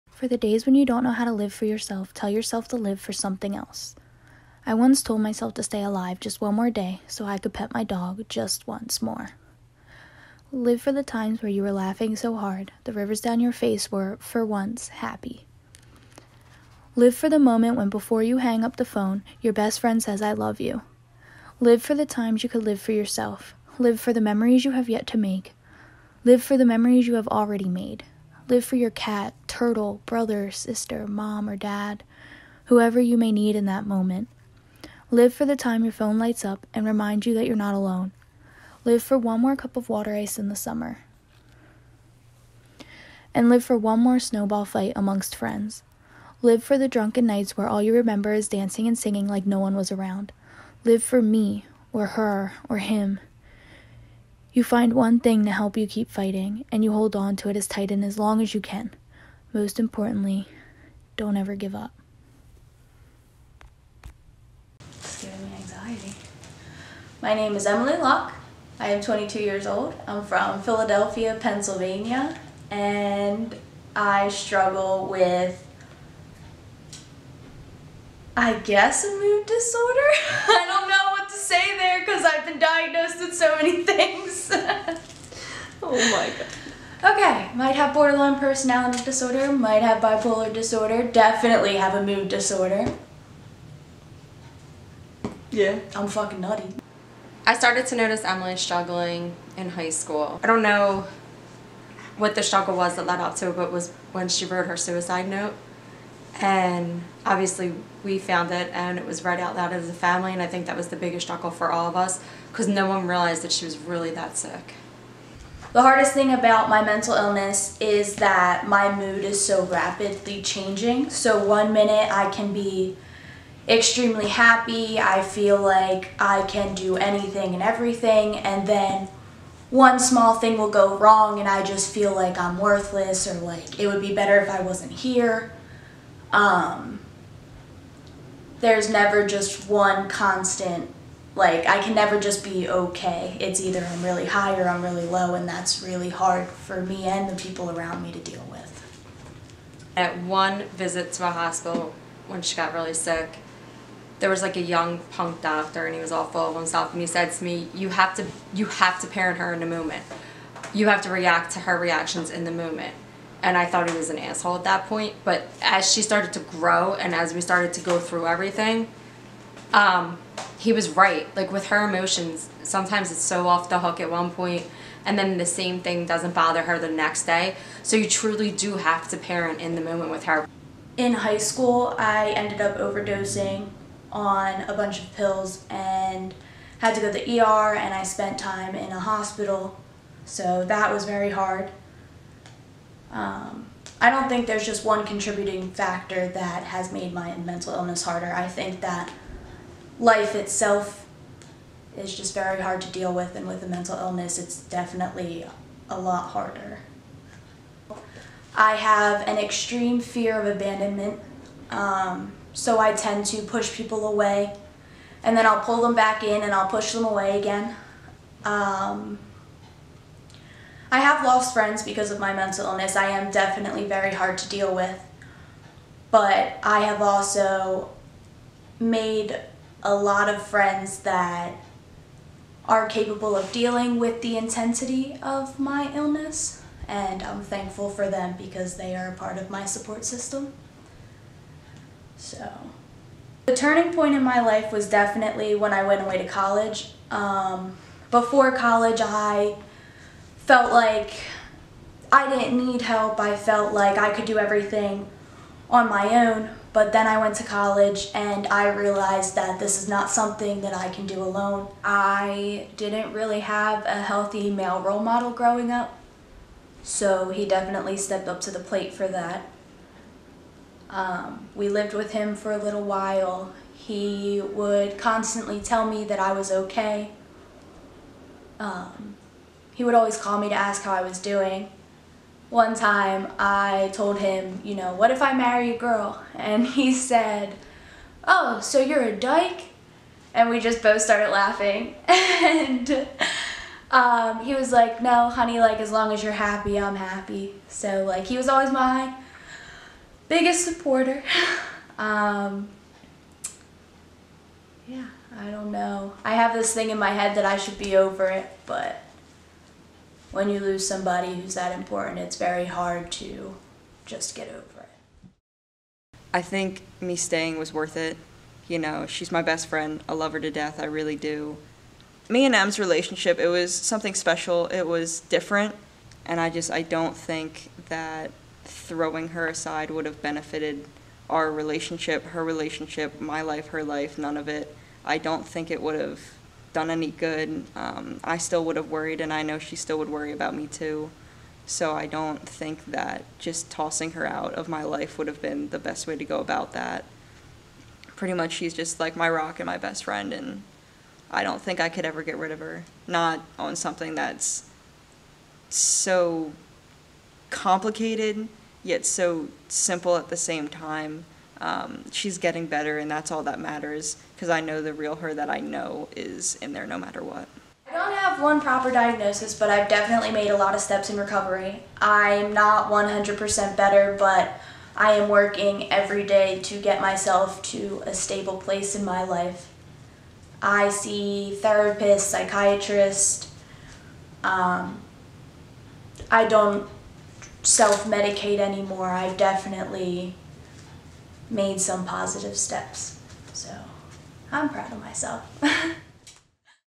My Misdiagnosed Life
A documentary on Emily Lauck and her struggles with being misdiagnosed. She has been misdiagnosed with different mood disorders, including borderline personality disorder and bipolar disorder. This documentary shows you the downfalls she has conquered and the uplifting moments that keep her fighting the never ending battles she faces day to day.